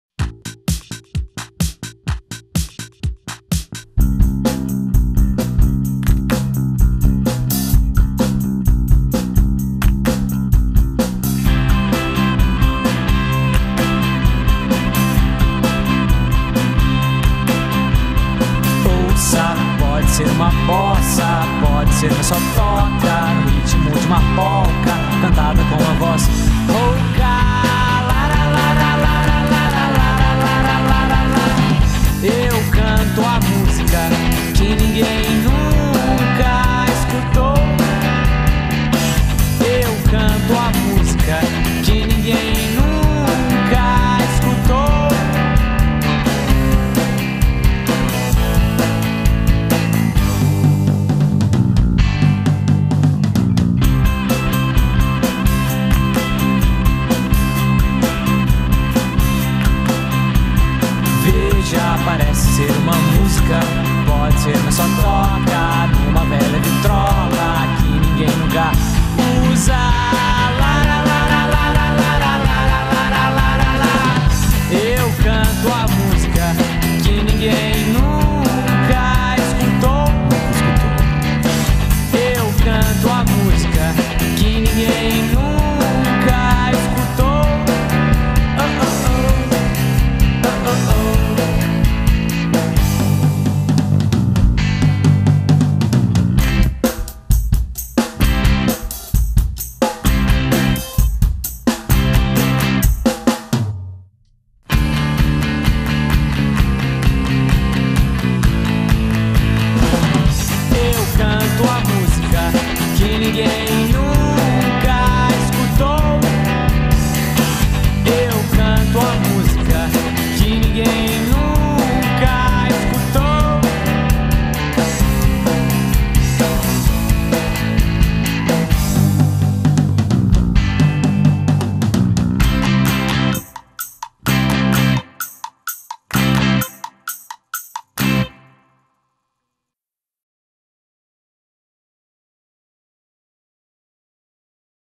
Verde Vida BaZar Pamplona - Show
Show do Bazar Pamplona na Casa Grande, Nova Olinda - CE